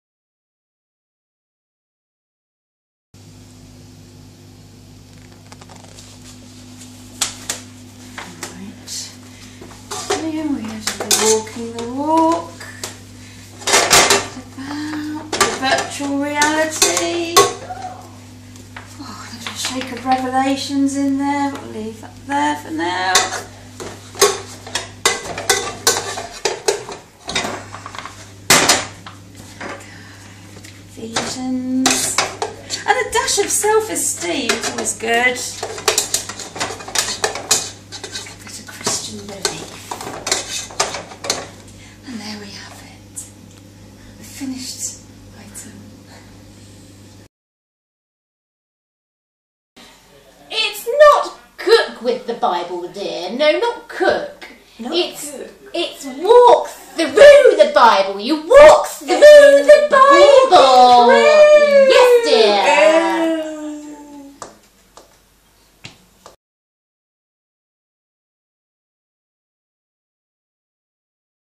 WTTB Cook
A short invitation to the Walk Through The Bible Seminar at Broadwater Baptist Church... it's not cook with the Bible!